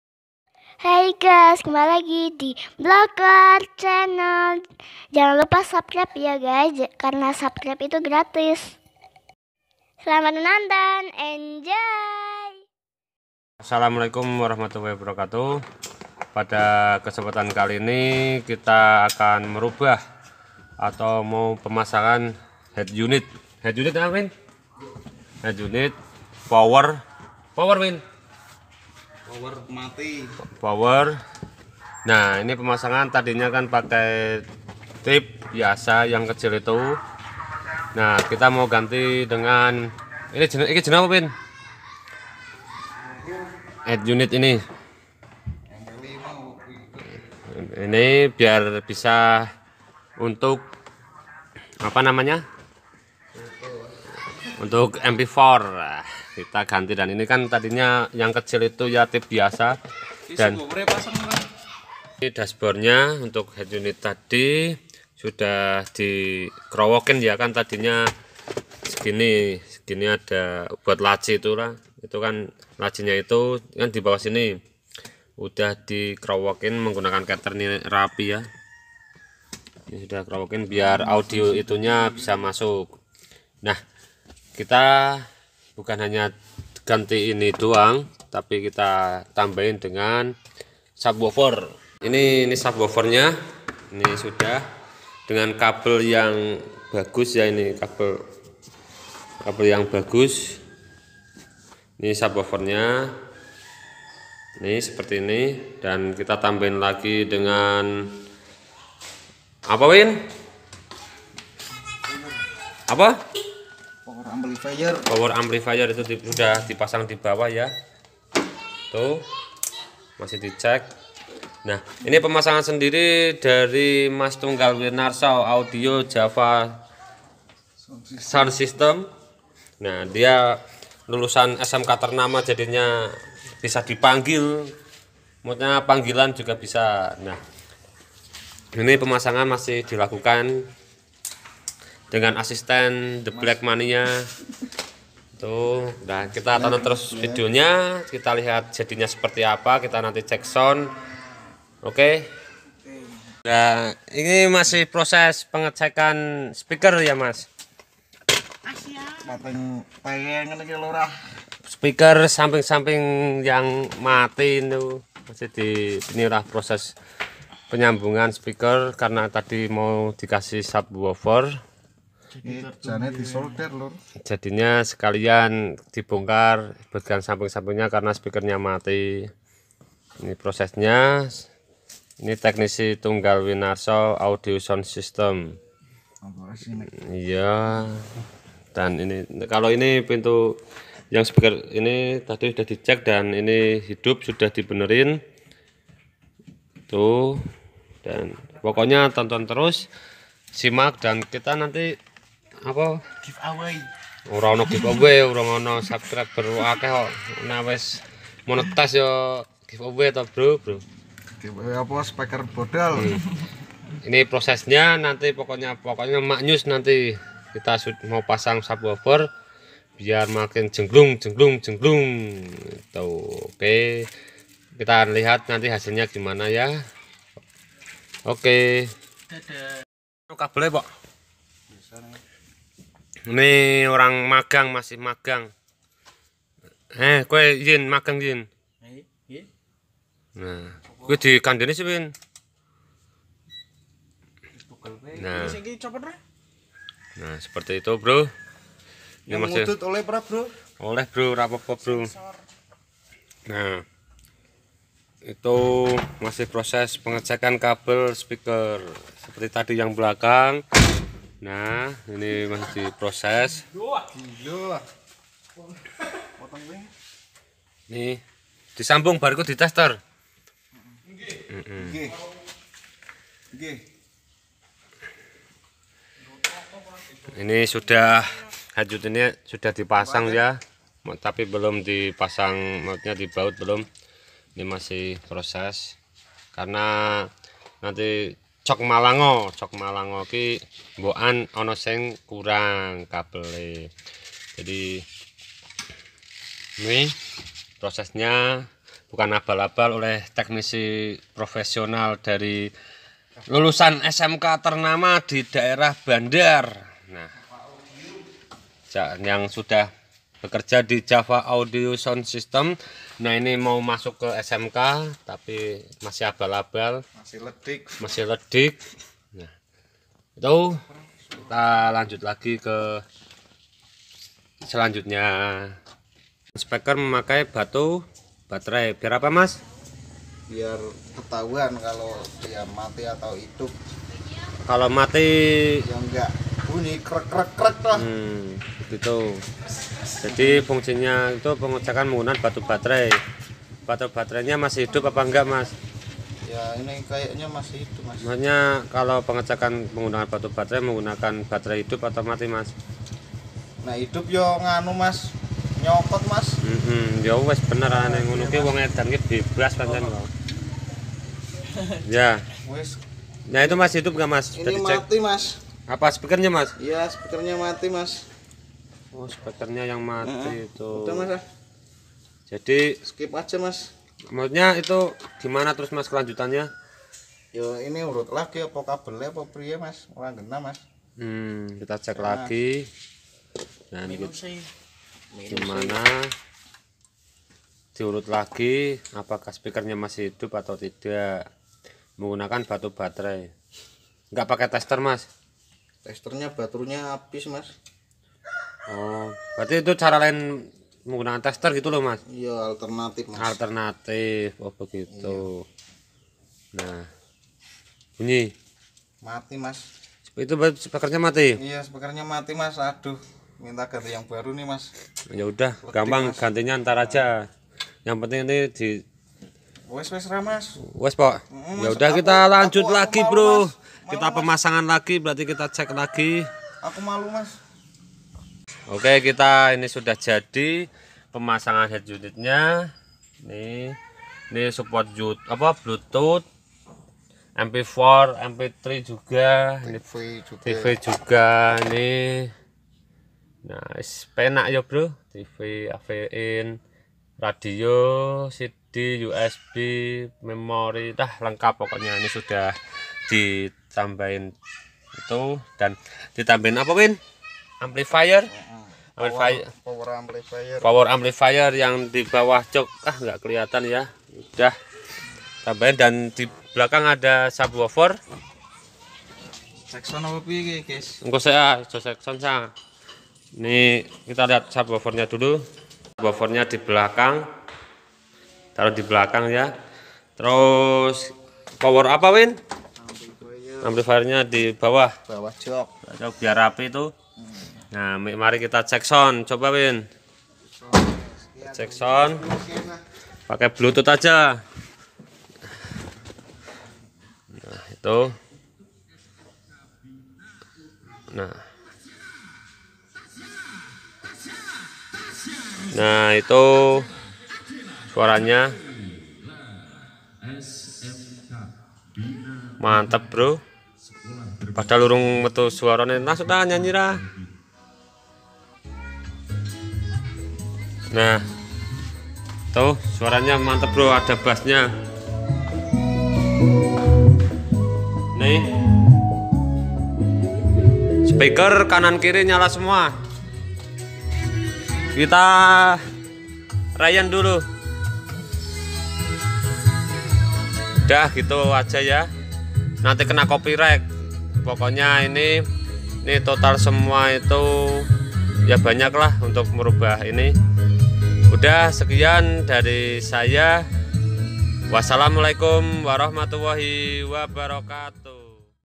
Mengganti head unit double din all new avanza / mpleker chanel
pada vidio ini merupakan proses penggantian head unit all new avanza 2012, gimana prosesnya??  simak vidionya sampai habis